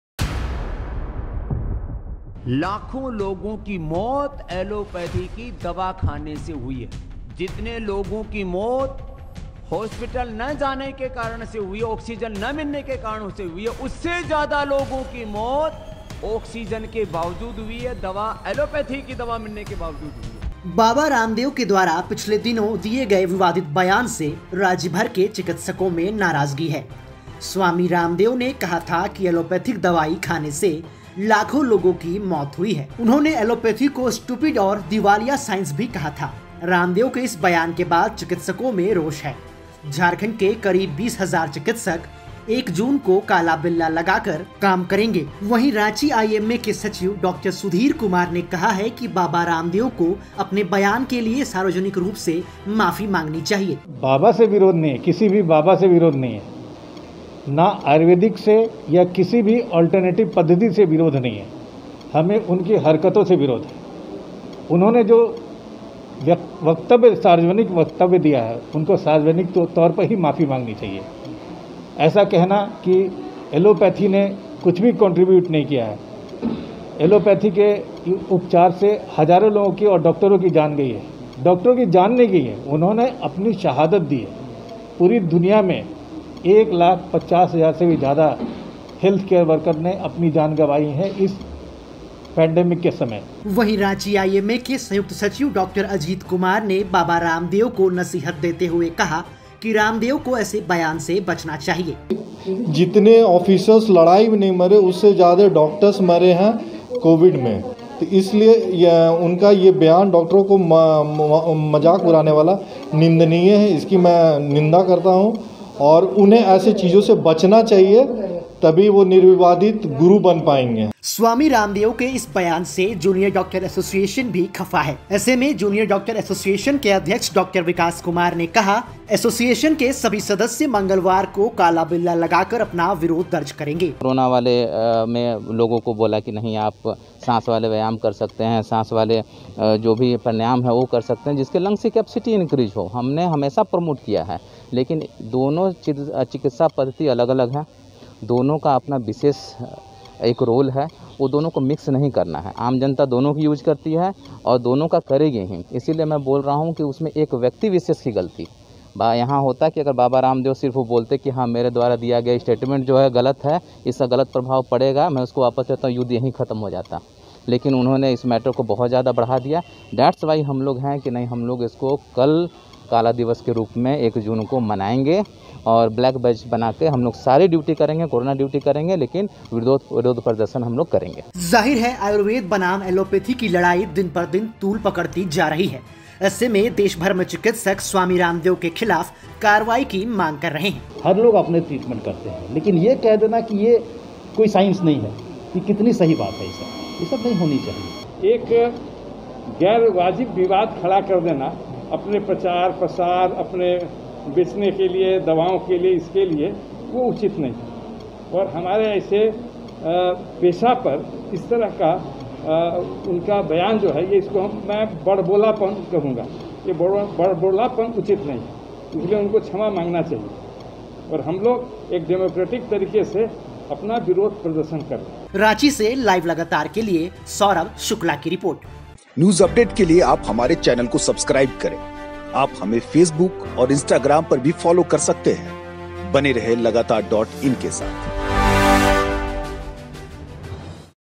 बाबा रामदेव के खिलाफ गोलबंद हुए चिकित्सक, 1 जून को काला बिल्ला लगाकर करेंगे काम II Live Lagatar II
Ranchi - बाबा रामदेव के खिलाफ गोलबंद हुए चिकित्सक, 1 जून को काला बिल्ला लगाकर करेंगे काम II Live Lagatar II